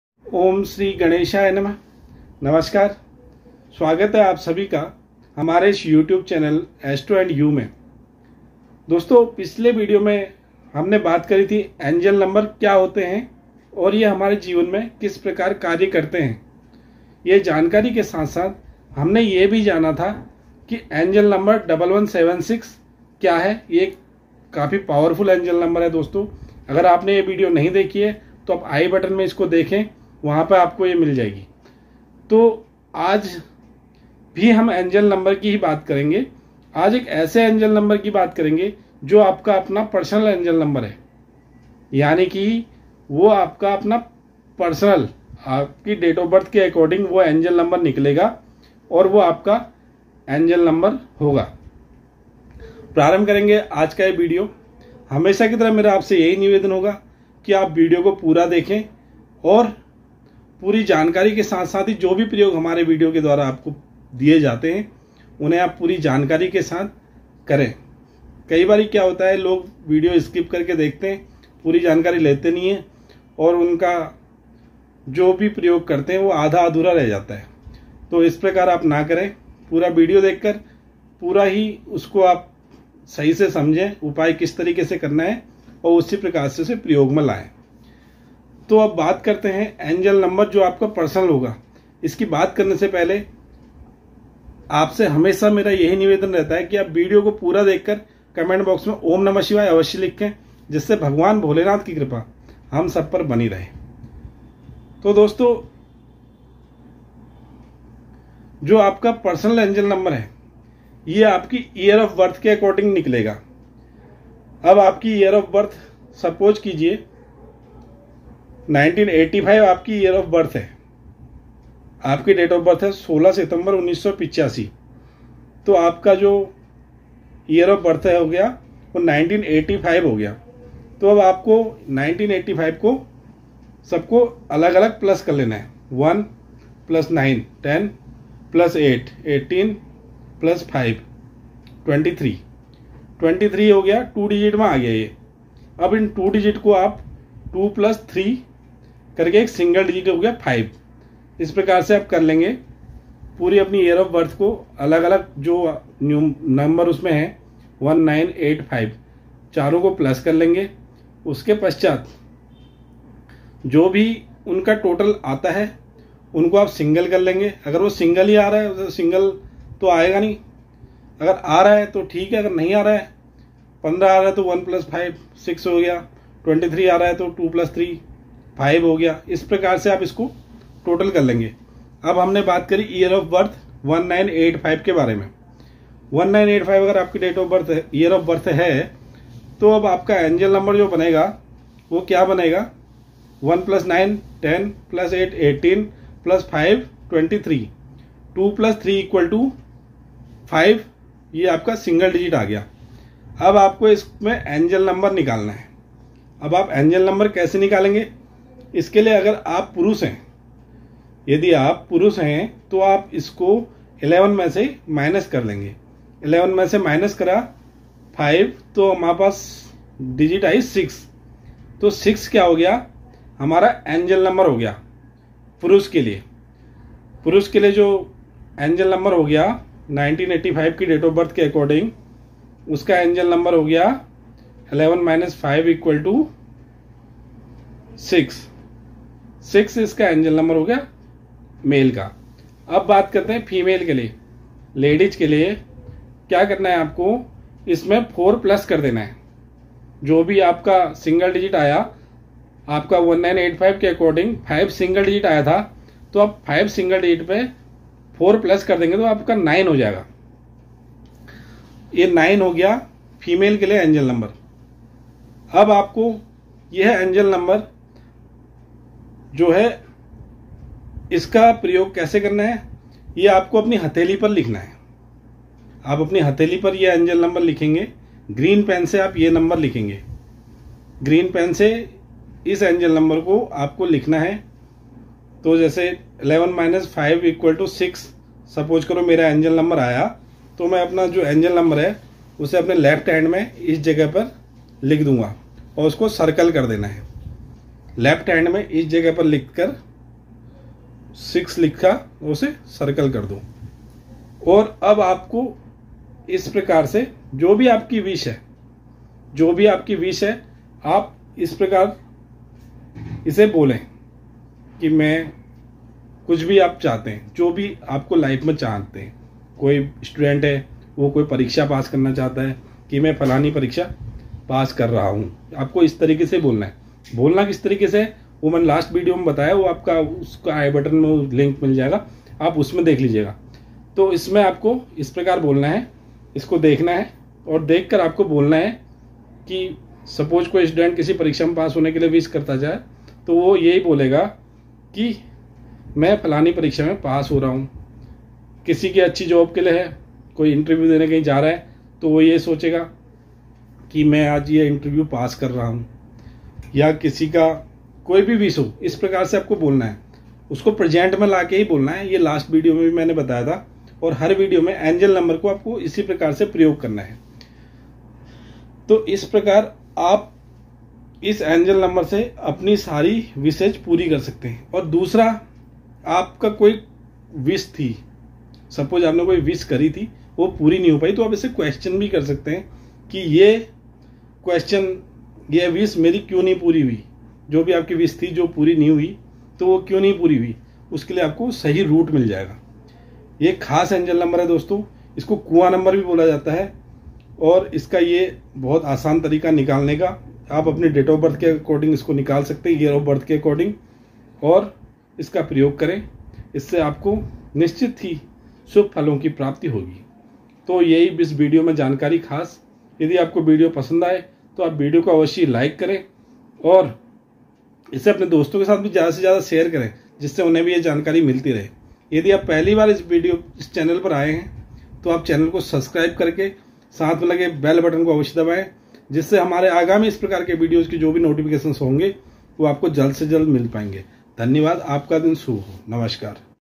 Angel Number | क्या है आपका Angel नंबर | खुद ही जाने अपना एंजल नंबर इससे होगी आपकी सब मनोकामना पूरी
नमस्कार दोस्तों
एस्ट्रो एंड यू चैनल में आप सभी का हार्दिक स्वागत है दोस्तों आज के वीडियो के माध्यम से हम जानेंगे एक ऐसे एंजल नंबर के बारे में जिसके प्रयोग से हम अपनी किसी भी विश् को पूरा कर सकते हैं वह नंबर है जो आप की डेट ऑफ बर्थ के हिसाब से निकाला जाता है

points discussed in this video :
अपना एंजल नंबर कैसे जाने
क्या है आपका अपना एंजल नंबर
एंजल नंबर किस प्रकार कार्य करता है
एंजल नंबर को कहां पर लिखें
डेट ऑफ बर्थ से अपना एंजल नंबर कैसे निकाले
ईयर ऑफ बर्थ से एंजल नंबर कैसे निकाले
जीवन में सफलता के लिए एंजल नंबर का प्रयोग कैसे करें
Angel नंबर कैसे सारी मनोकामना पूर्ण करता है